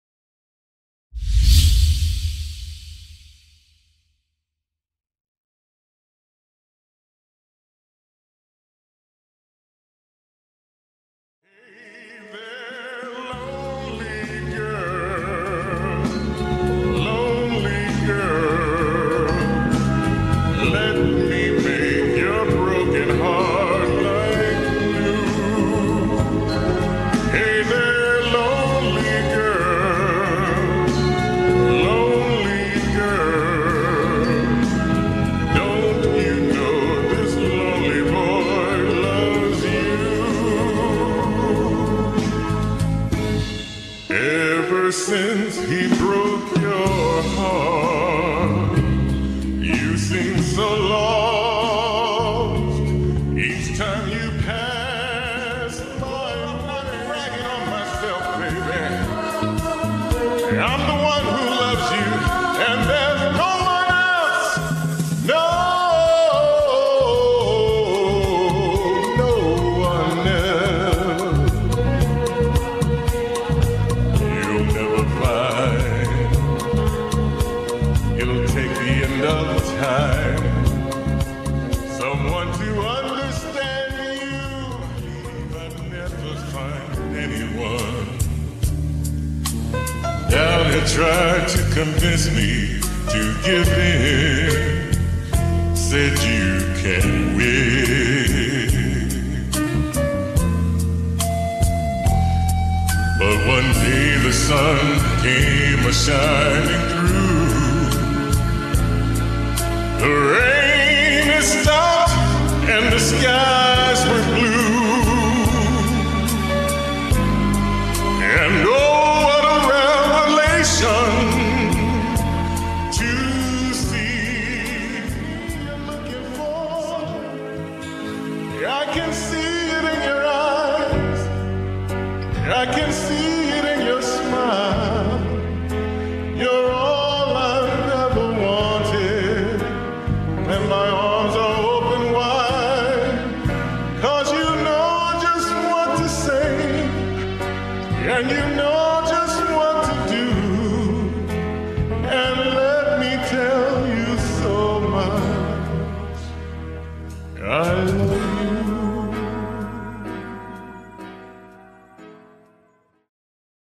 A live Karaoke Medley which includes a Lionel Richie hit
Just another display of Eric's vocal range and versatility.